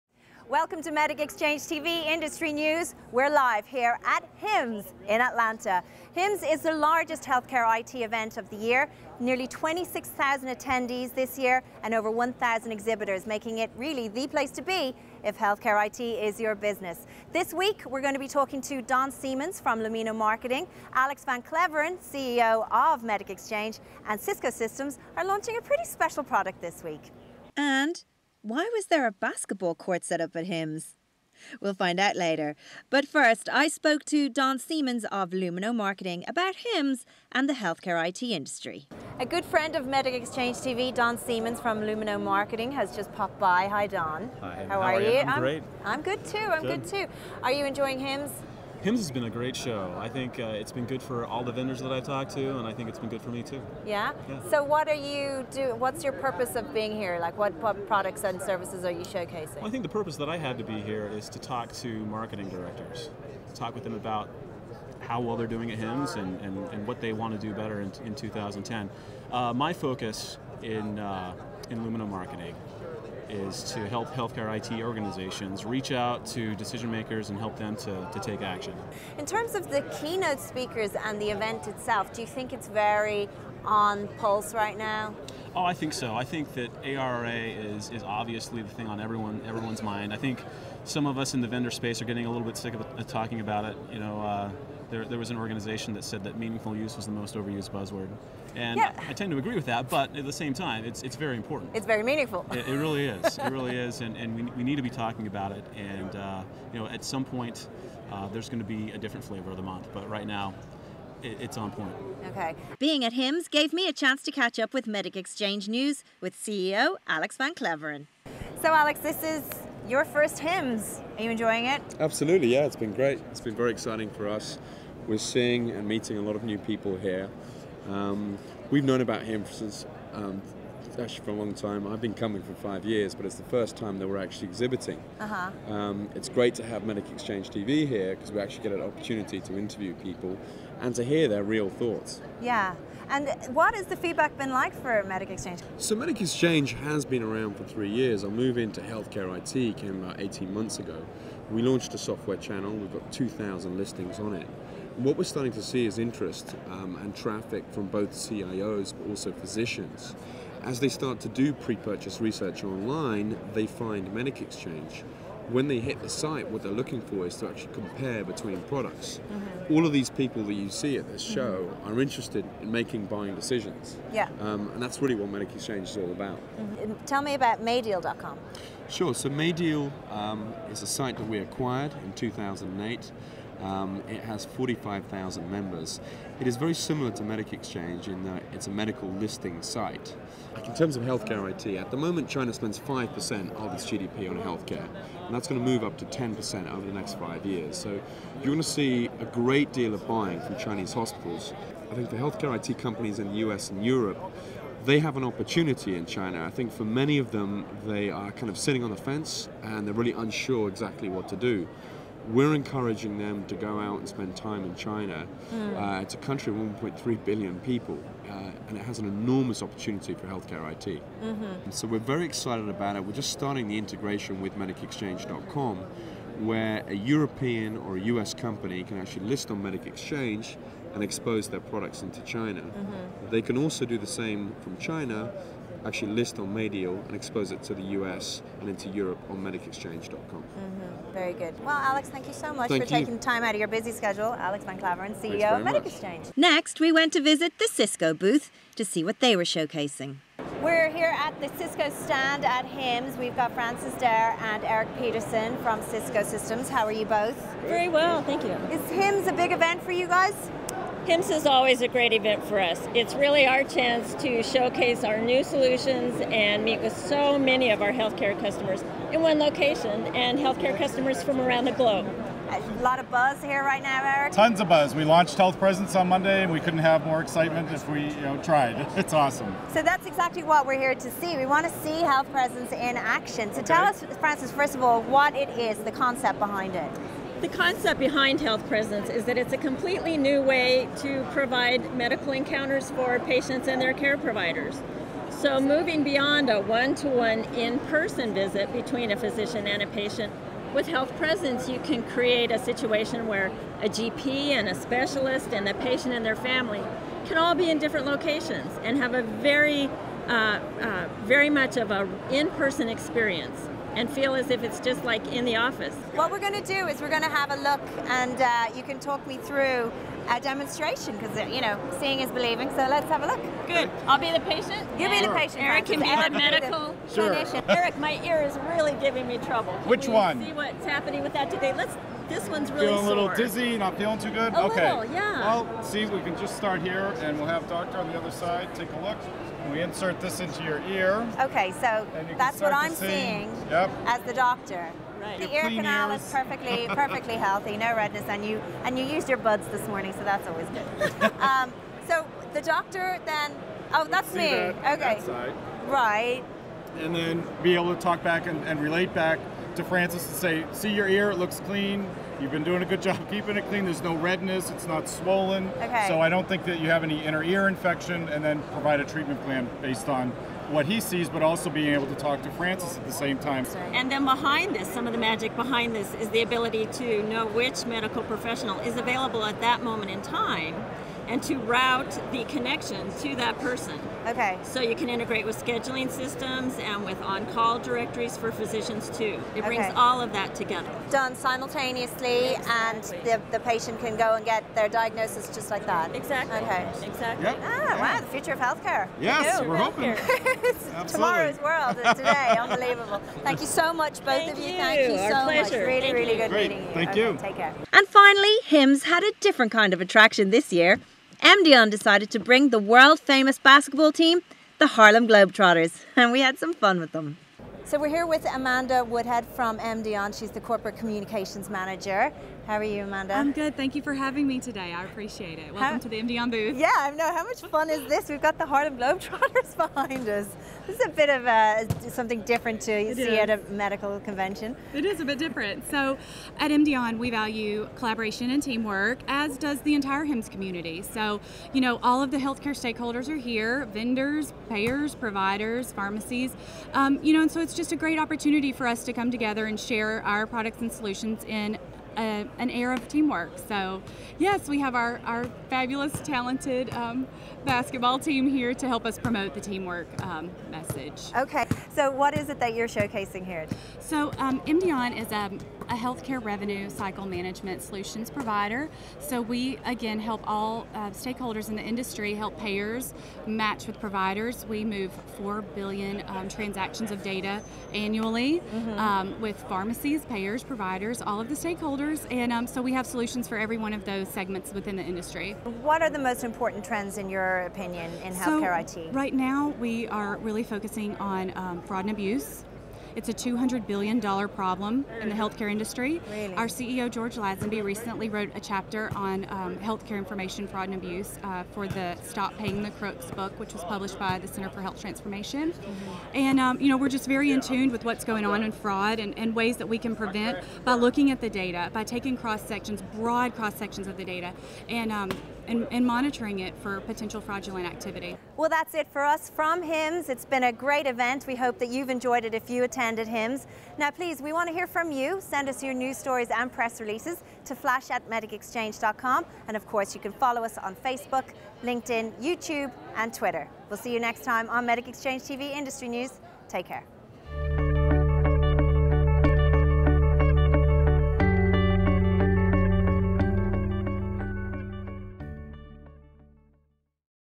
Industry News direct from HiMSS10, Atlanta, March 5 2010
See Don Seamons, Managing Principal at Lumeno Marketing, and Alex van Klaveren, CEO of MedicExchange, as they discuss the conference in Atlanta and share their insights into current industry trends.

Also, at Cisco's booth, Frances Dare, Director, Healthcare Consulting Practice of Cisco's Internet Business Solutions Group (IBSG), and Erik Peterson showcase the technology giant's innovative solution - Health Presence.

Finally, from the basketball court at Emdeon's booth, former stars of the world-famous Harlem Globetrotters demonstrate their skills while we catch up with Communications Manager, Amanda Woodhead.

- throughout the week to view the 75+ interviews we filmed with senior industry executives and thought leaders.